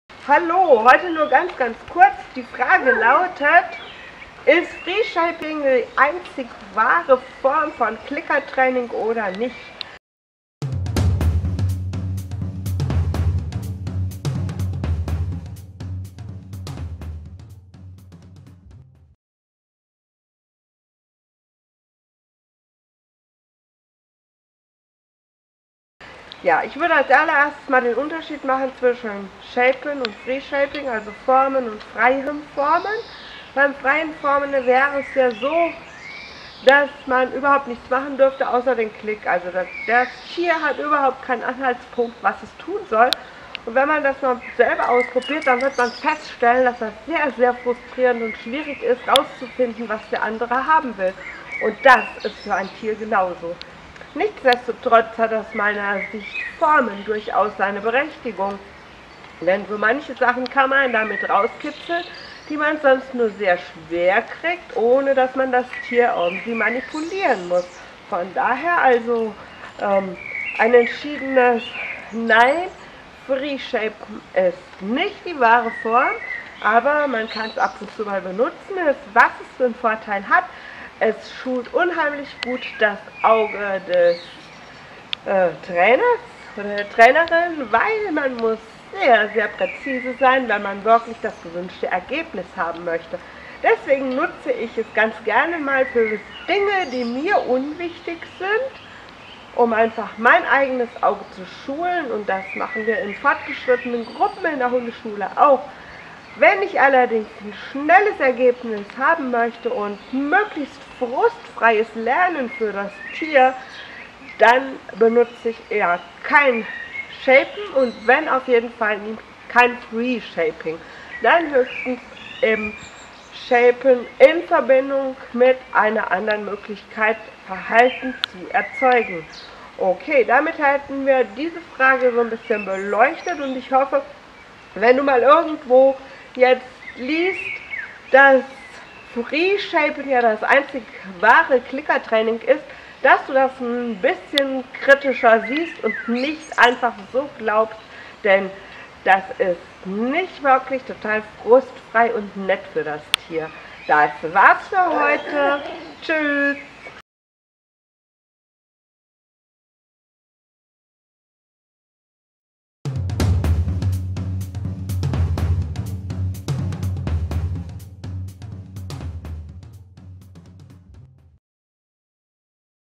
Mein lieber Hund KRITISCH - Ist freies Formen das einzig wahre Clickertraining?
Nur, wer frei formt, macht wirklich richtiges Clickertraining.
Das wird immer mal wieder so erzählt oder man kann es irgendwo nachlesen.

Warum du das etwas kritischer hinterfragen solltest, erfährst du in der heutigen Episode.

Hundeschule Mein lieber Hund
Claudia Hußmann
Ziegenhainer Str. 52
34576 Homberg (Efze)